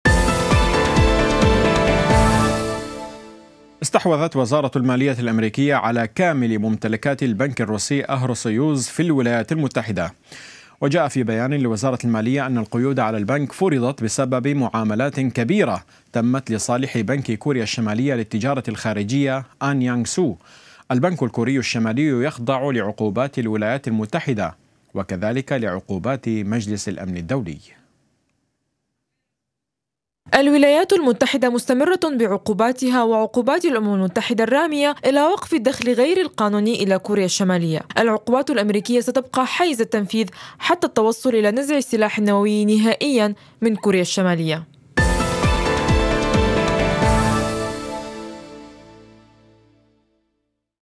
وزارة المالية الأمريكية تصادر ممتلكات بنك روسي تعاون مع كوريا الشمالية
استحوذت وزارة المالية الأمريكية على كامل ممتلكات البنك الروسي "أهروسويوز" في الولايات المتحدة.